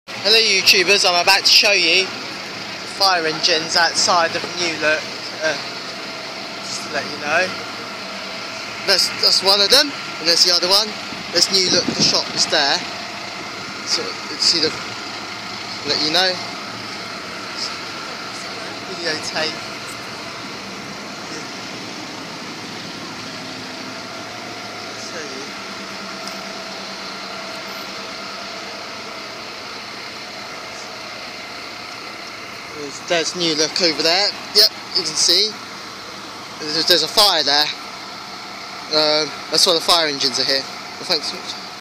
Fire engine s outside of newlook Guildford
There is real fire today.